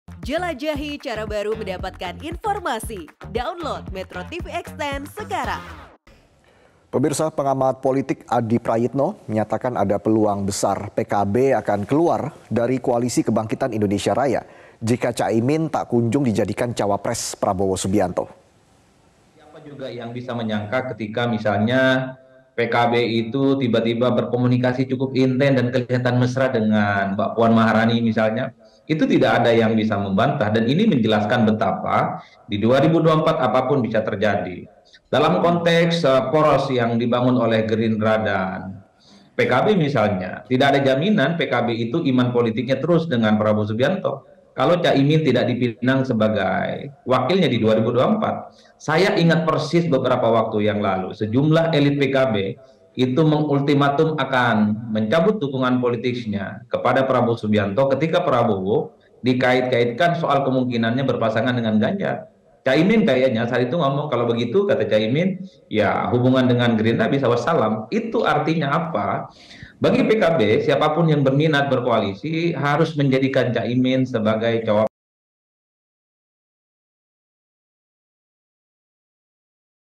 Tak Kunjung Jadi Bacapres, PKB Bisa Pisah Dengan Gerindra
MetroTV, Pengamat Politik mengatakan ada peluang besar PKB akan keluar dari koalisi kebangkitan Indonesia raya jika tak kunjung Cak Imin dijadikan Cawapres.